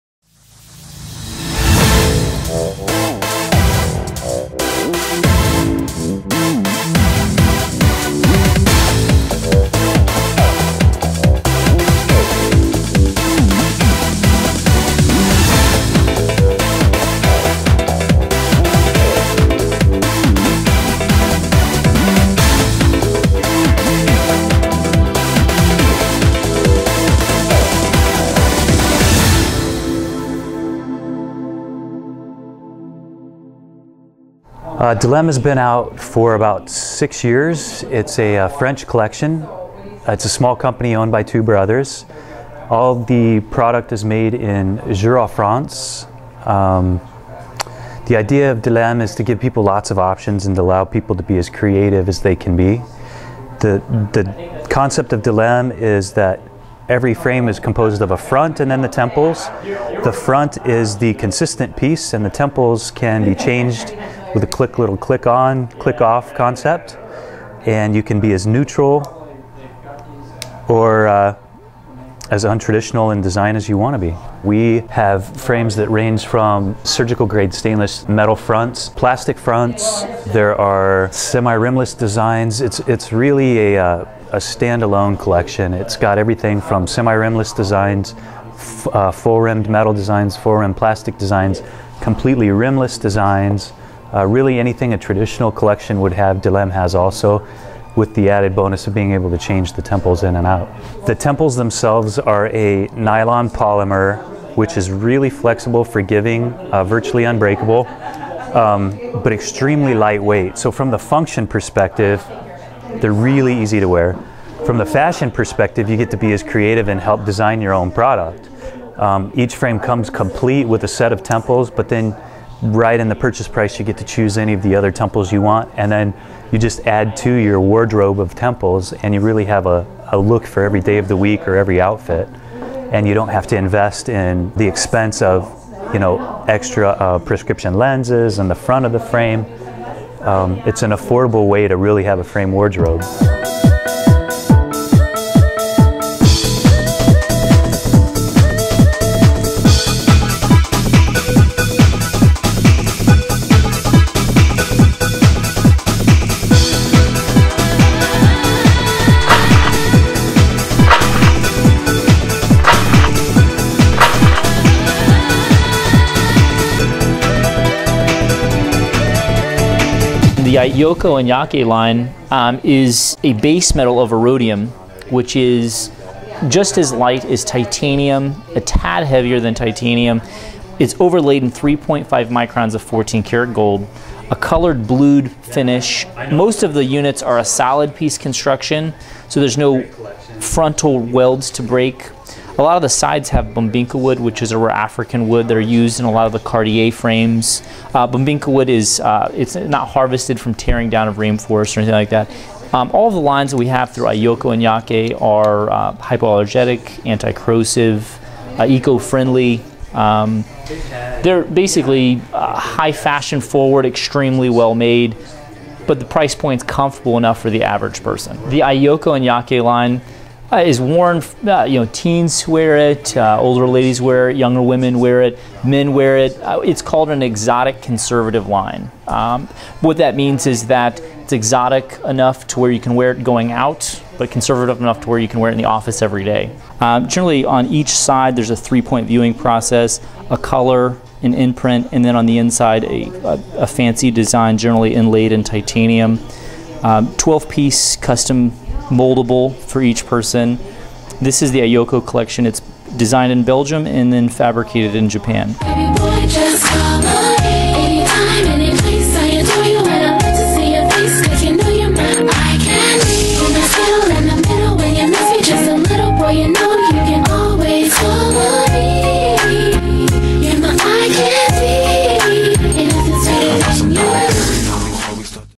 delimtrunkshow.wmv
Eyecandy Eyewear in Delfield wanted us to create a fantastic video for their trunk show and we came up with this...they LOVED it!!!